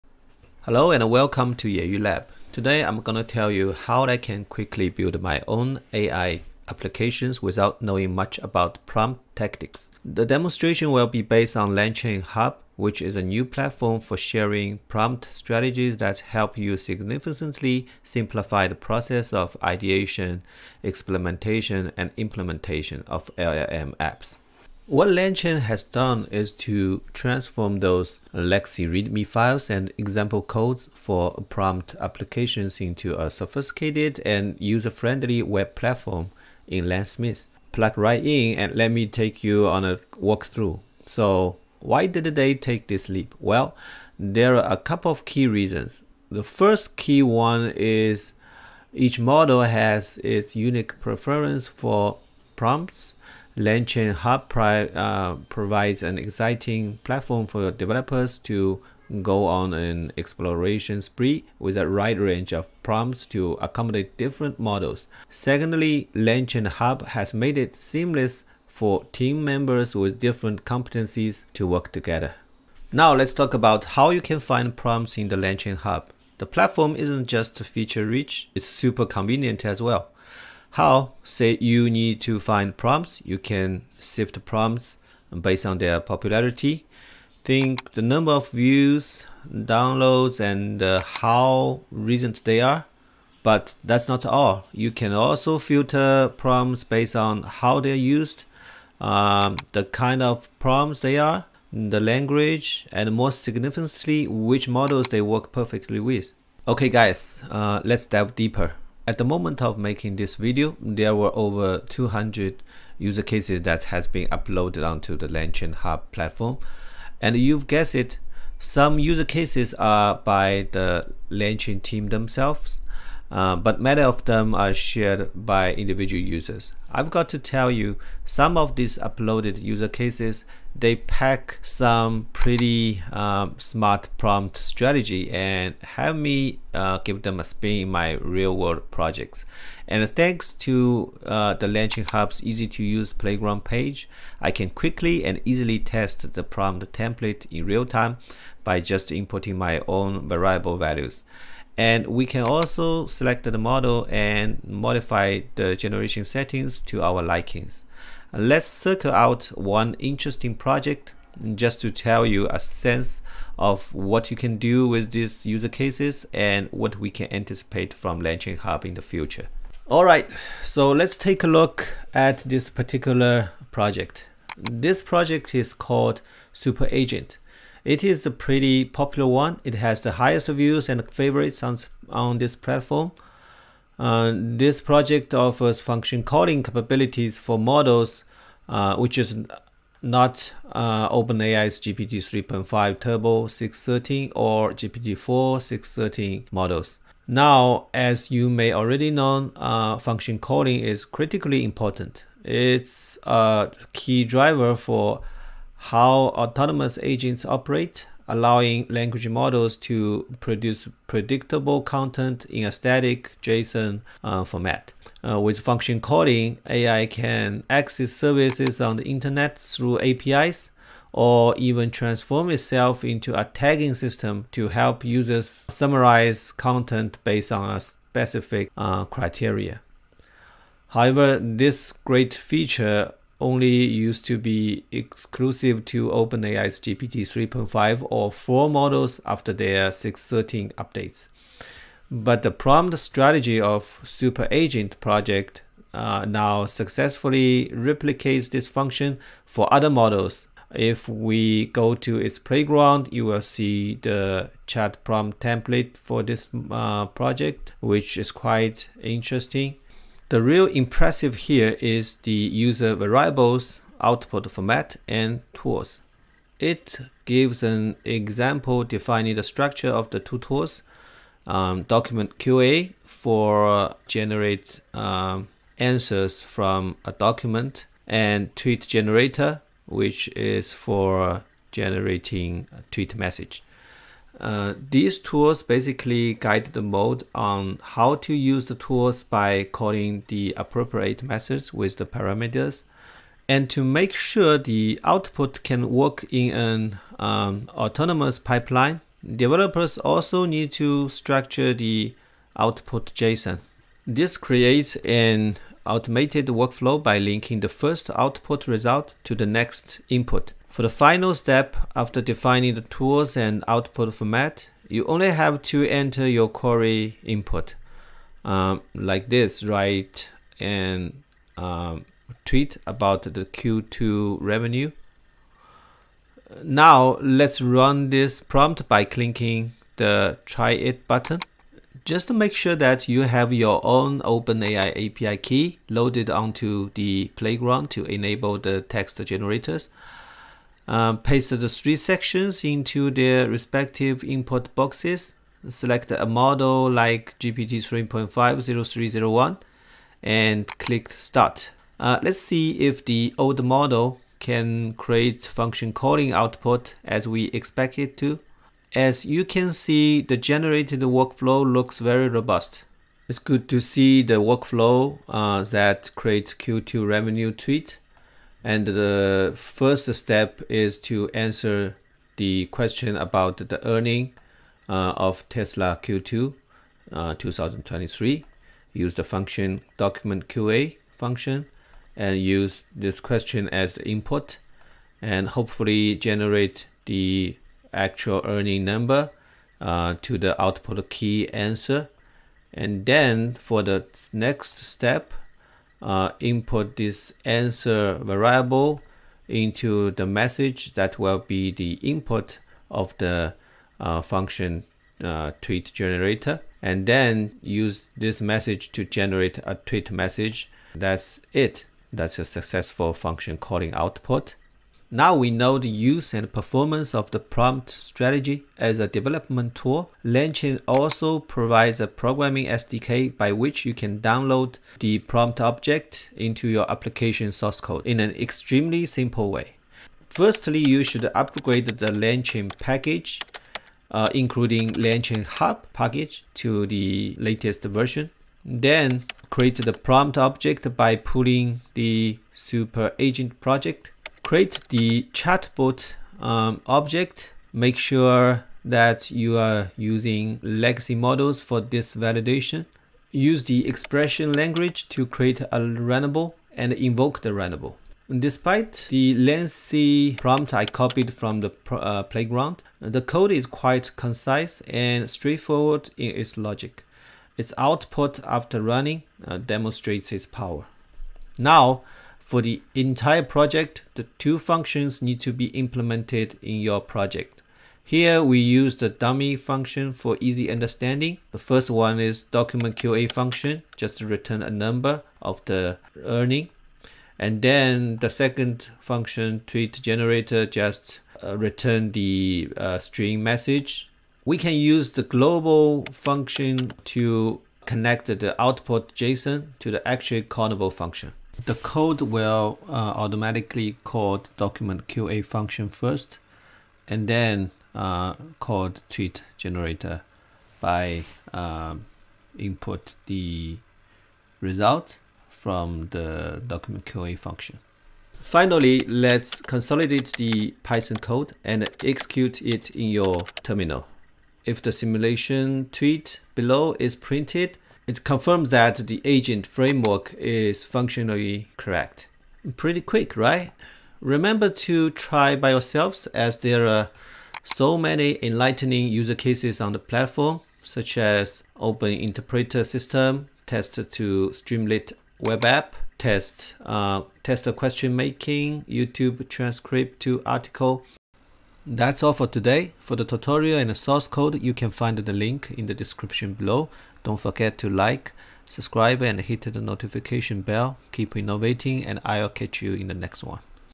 LangChain Hub - Hundreds of Prompt Templates that Streamline Your LLM App Development
In this video, we will look at the Langchain Hub platform where LLM developers share various prompt templates covering interesting user cases. We will discuss the entire implementation of a Function Calling agent by using the prompts strategy from Langchain Hub.